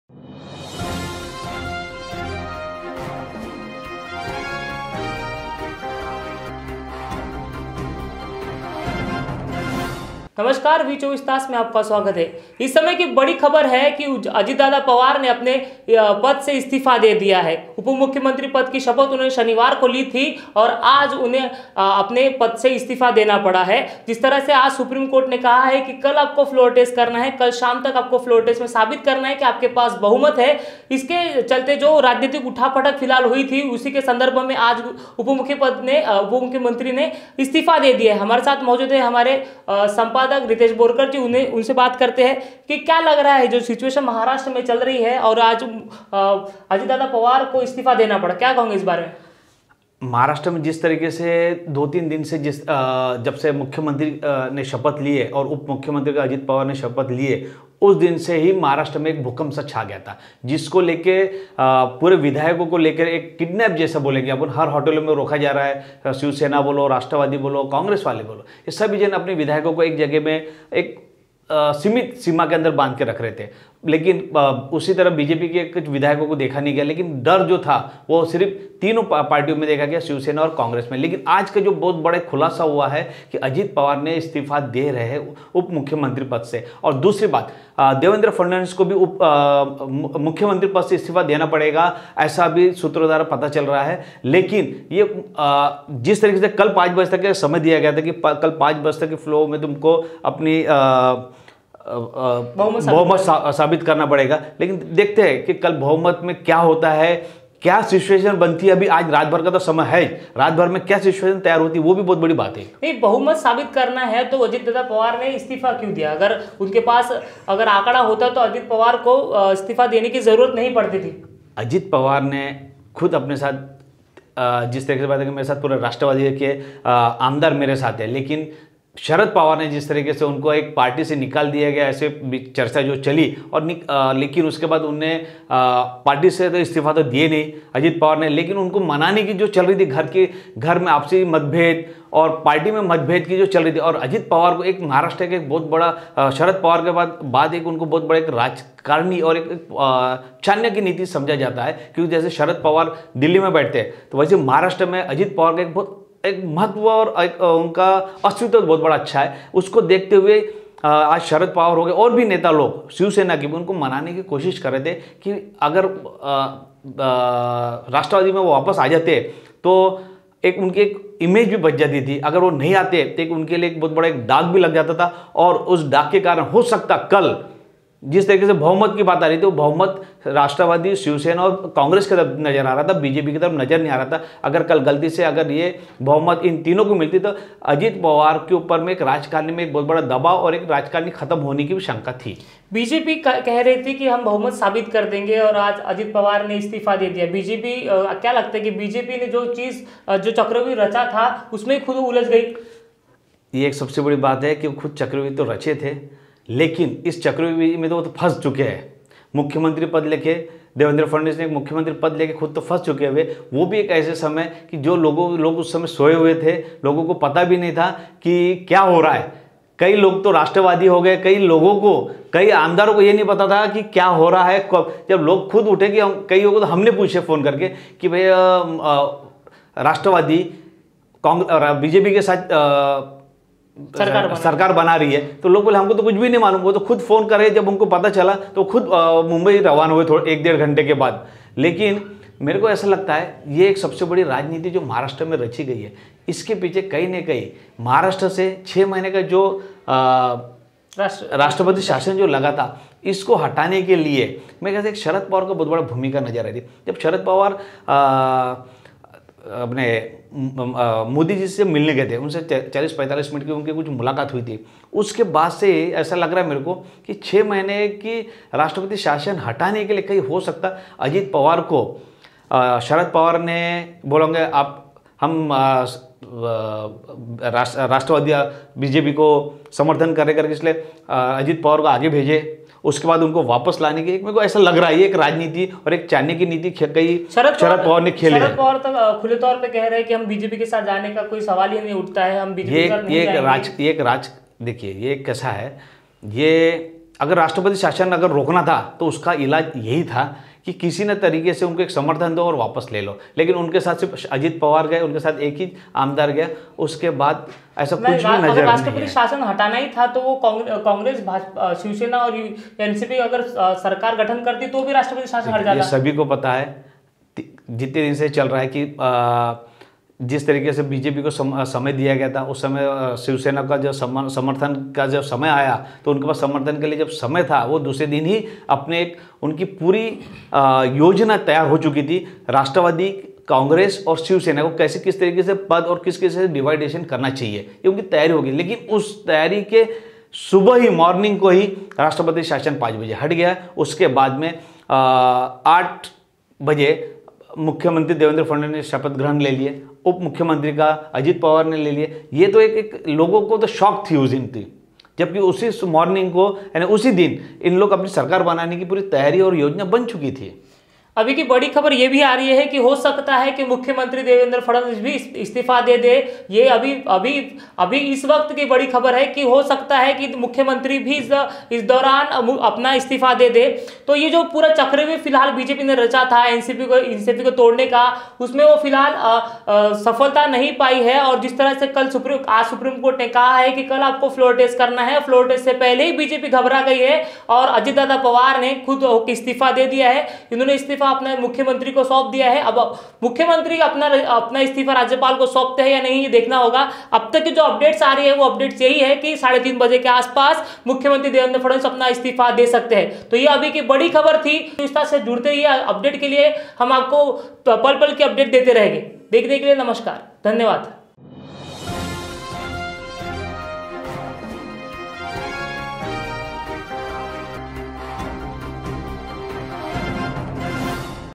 अब किसकी बनेगी सरकार महाराष्ट्र मे ?I V24 Taas I 26 Nov 19
अजित दादा पवार ने दिया उपमुख्यमंत्री पद से इस्तीफा